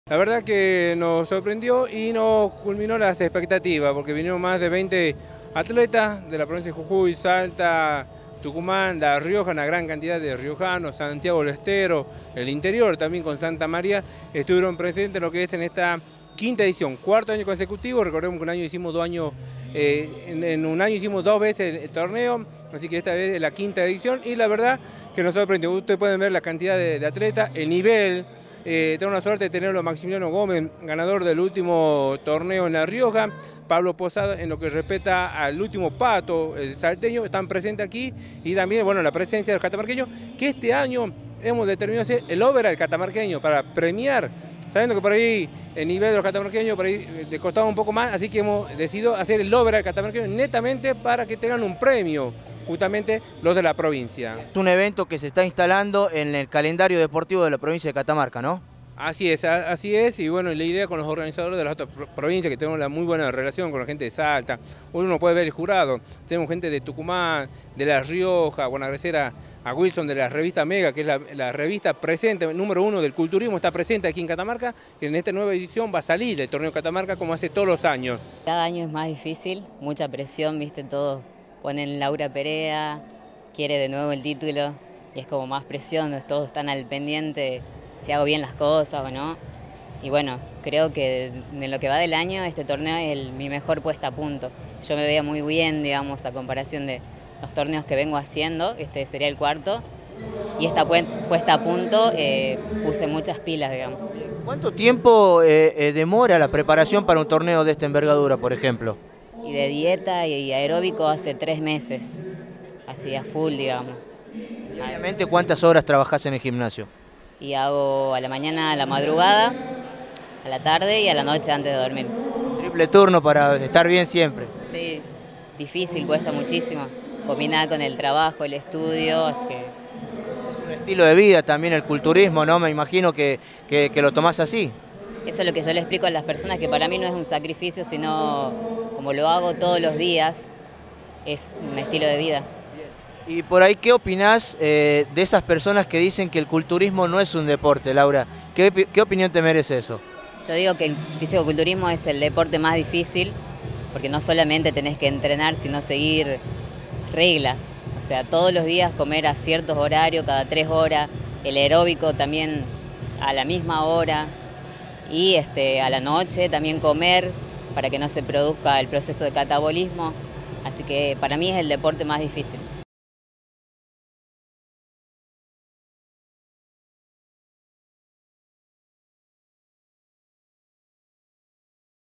Notas Fernando Zamiruk y Laura Perea
5° Torneo de Fisicoculturismo de Catamarca